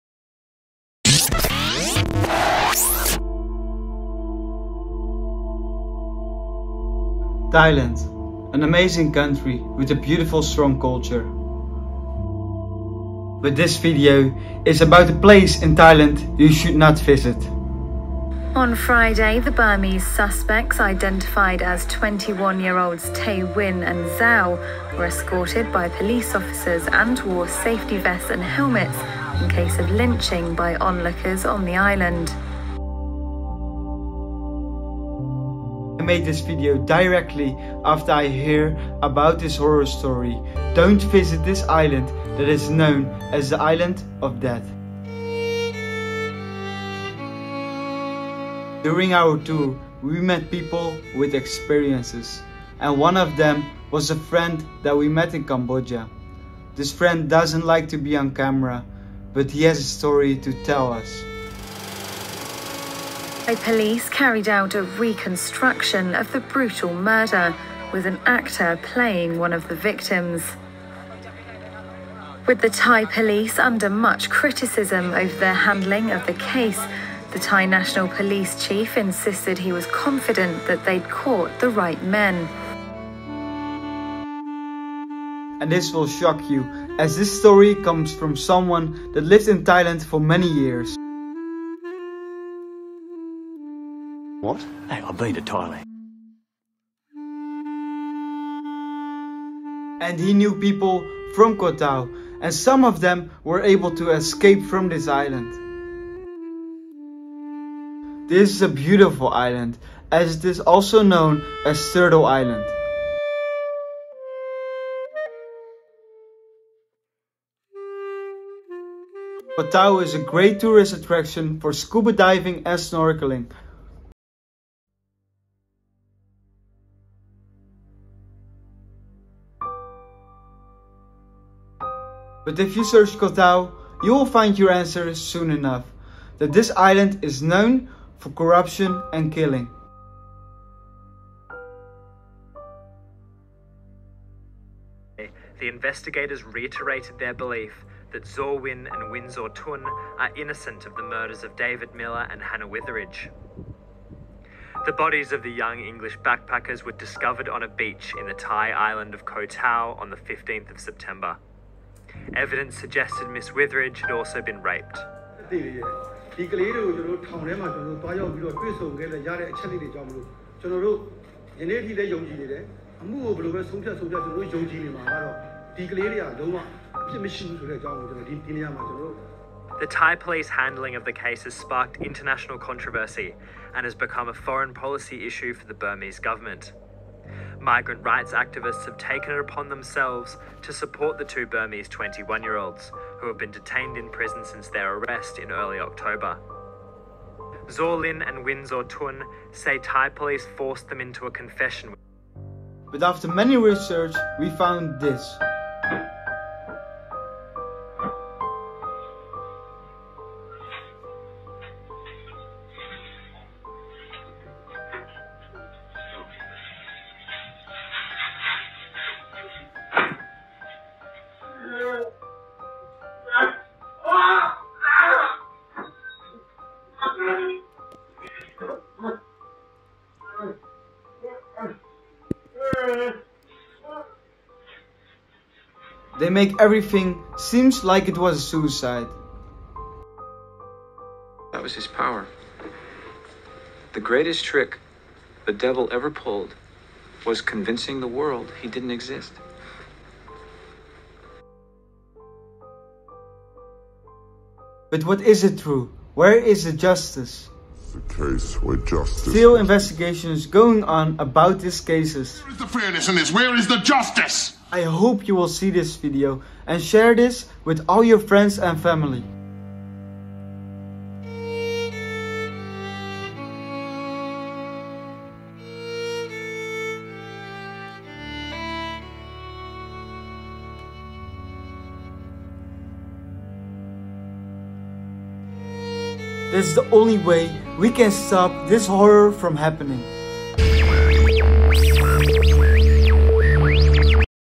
DANGEROUS ISLAND!💀🏝️
This video is based on true stories and real fragments about KOH TAO!

I want to give big shoutout to @KOHTAOMURDERSDEATHISLAND Because of Ian I was able to have more information about these cases on Koh Tao!🙌

I made this video to warn all of you travelers out there for this islands curse as this was a prison before it became a turtle paradise!

Save the beauty of Thailand by traveling to other places in Thailand.
 I don't want to put Thailand as a country in a bad day light but this goes about a specific island  in Thailand that is known for the corruption!

This island is in the wrong hands from corrupt police and general!

I hope this will change in the future so we could all visit this paradise!

(WARNING! this video contains violent video fragments)